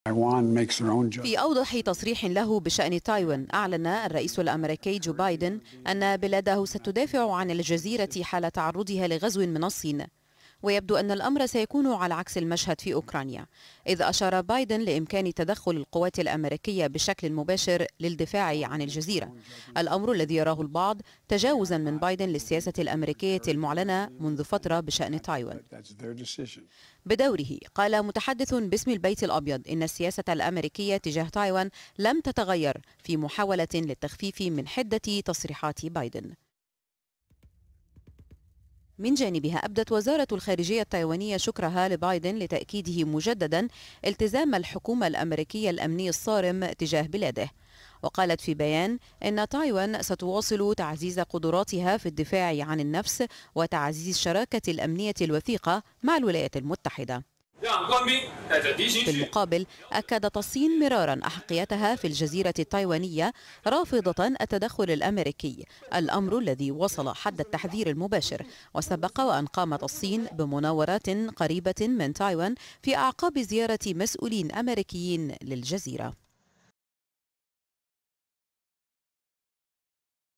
بايدن يؤكد دفاع أمريكا عن تايوان حال حدوث غزو من الصين والبيت الأبيض يحاول التهدئة
اعتبرت وزارة الخارجية الصينية أن تصريحات الرئيس الأمريكي جو بايدن حول تايوان انتهاك خطير لسياسة واشنطن حيال الجزيرة.
وكان الرئيس الأمريكي قد أكد أن القوات الأمريكية ستدافع عن تايوان في حال تعرضها لغزو صيني وذلك في أوضح تصريح له حتى الآن بشأن هذه المسألة.
من جانبها أثنت تايوان على تصريحات الرئيس الأمريكي, مؤكدة رغبتها في تعزيز دفاعاتها والتعاون الأمني مع الولايات المتحدة.